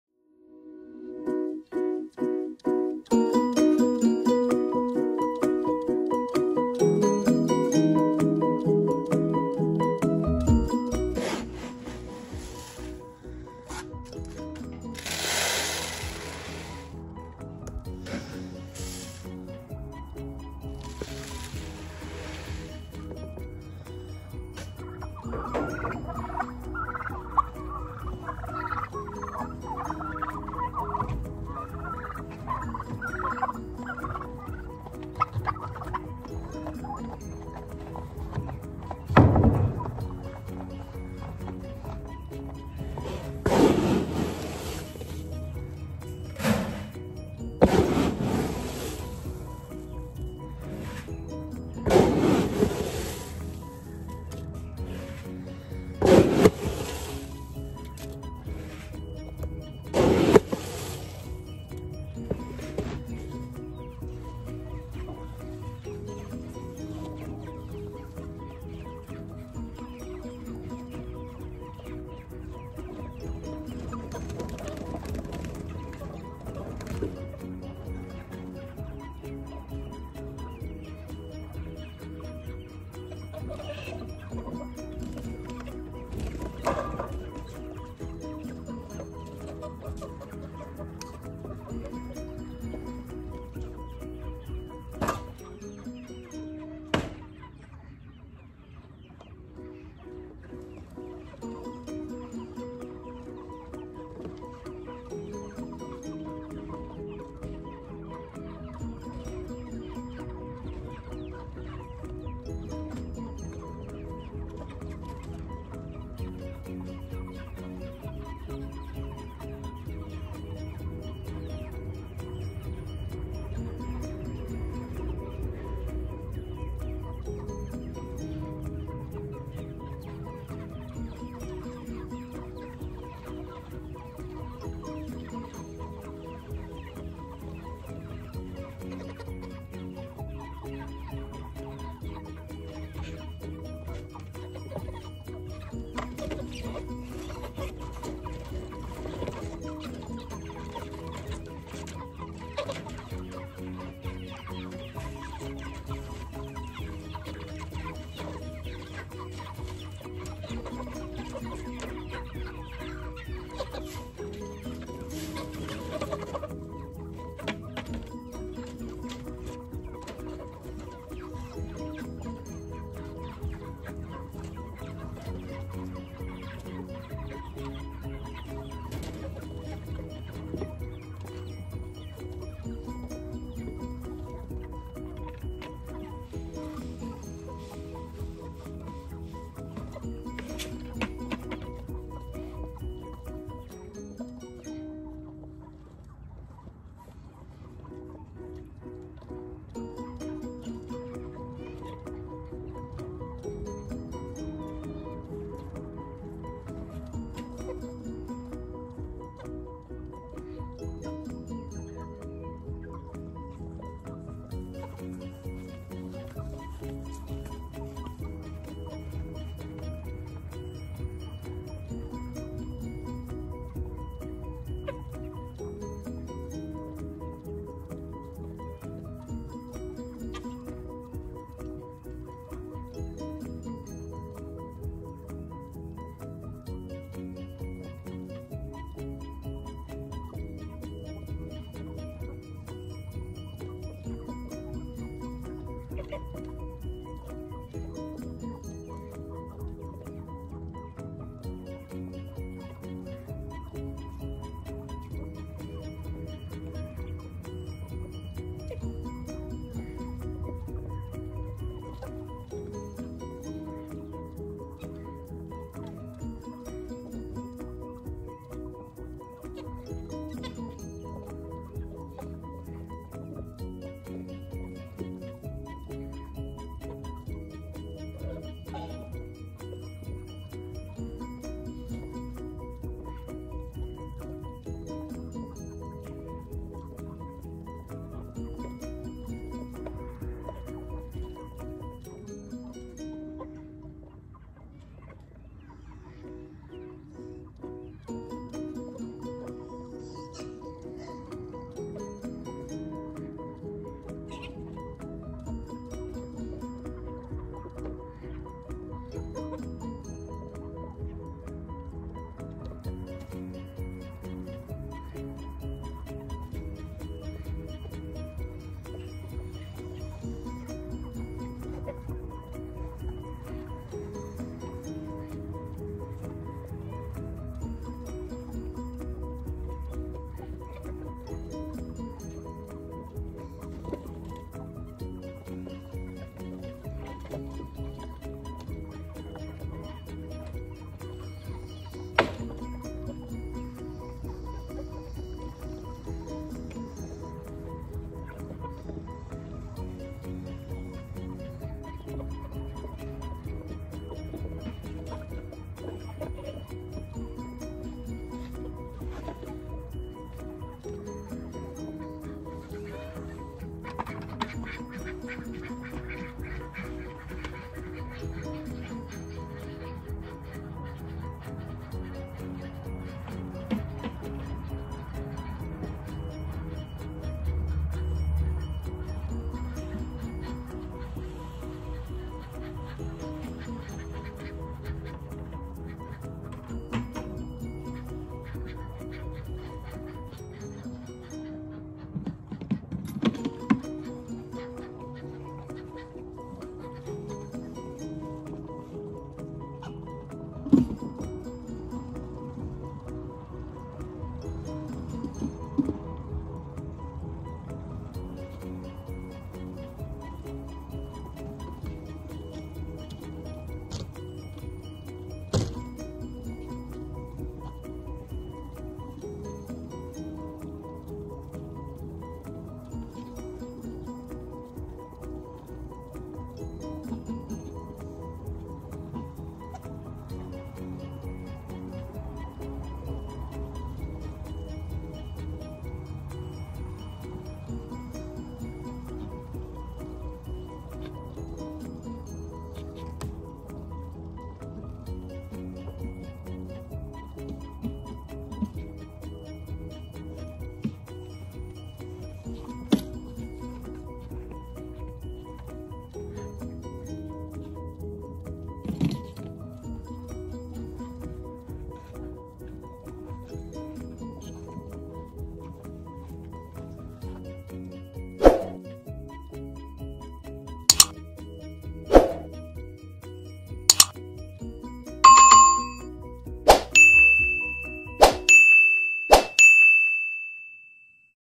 Chore time
Come along with me as i do some of the chores on the homestead. We have to feed pigs, chickens, turkeys and meat chickens every morning and night. Come along and enjoy.